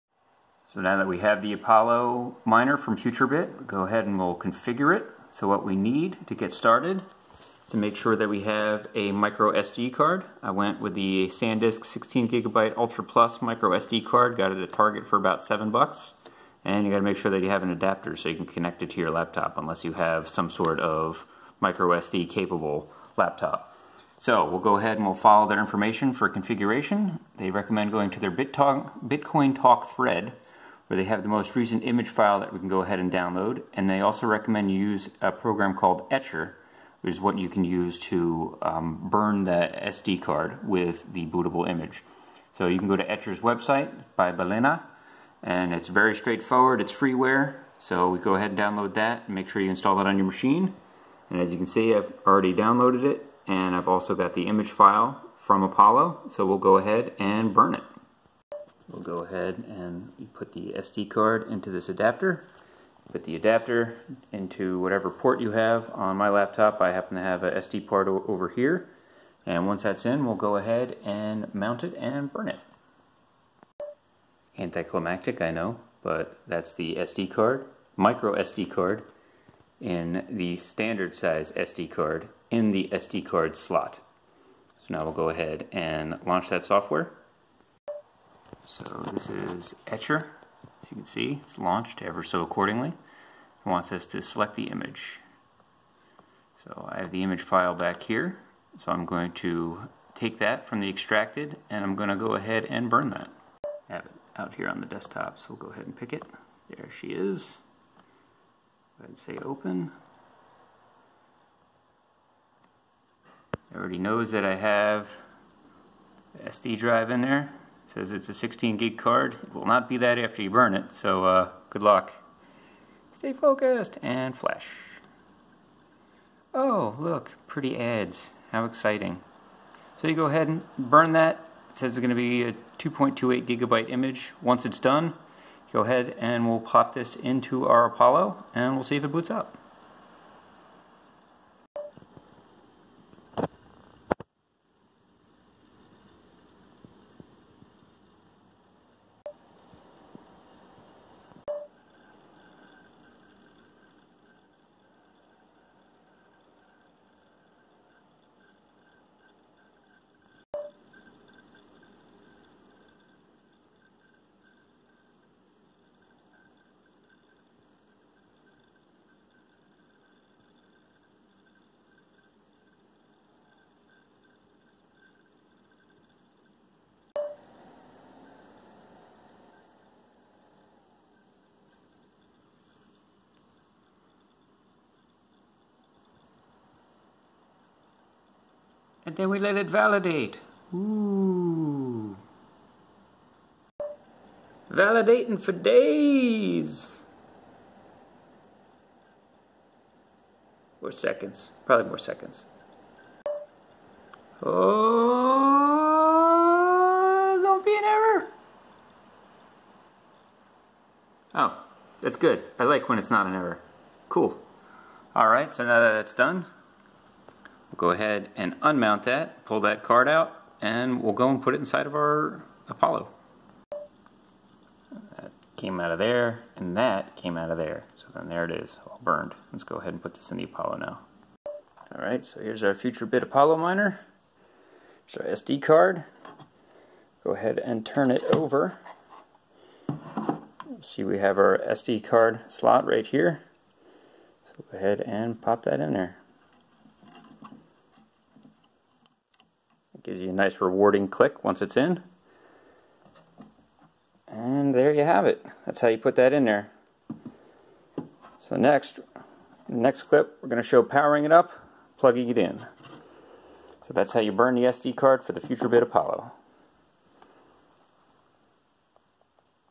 FutureBit Apollo ASIC Software Image Burn Guide
Following the best practice guidance from FutureBit directly, we go through the step by step of downloading, burning, and installing the needed software to get our Apollo ASIC going.

LITECOIN: MQScfHtyySfMqd3a4pLuWBuiFiNJoiQEsE
RIPPLE: rE9HrF1Cc5JKozEtGPaNHEyeuJ1HrA57fi
BITCOIN: 36CCdZVtWYovKhvFXWpnWmvxhuNNP6y92n
0x: 0x1aae7b7fcFF5E09Cf9C8cc1b1E30a00B70bB8c9c
DOGECOIN: D5H85ogQZdFdQoBdtBxtYez4FuQxAu8EVc